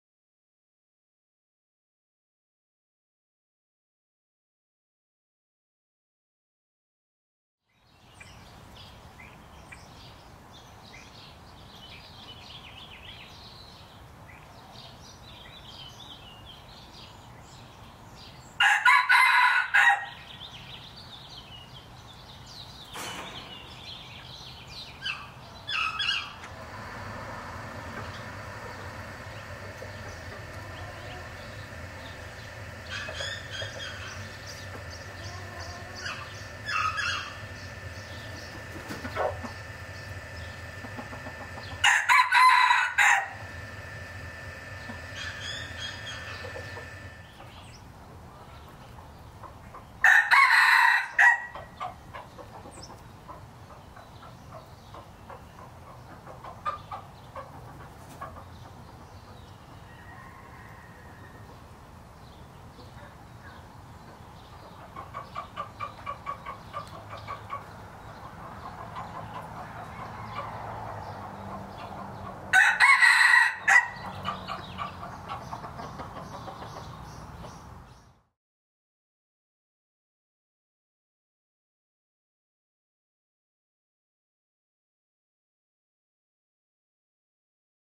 Kong's Red Junglefowls “Red” (2024)
Hello friends, this is one of my main breeders for 2024. He is named “Red,” is 3-years-old, and is a hybrid that is is approximately 75% (+/- 10%) pure.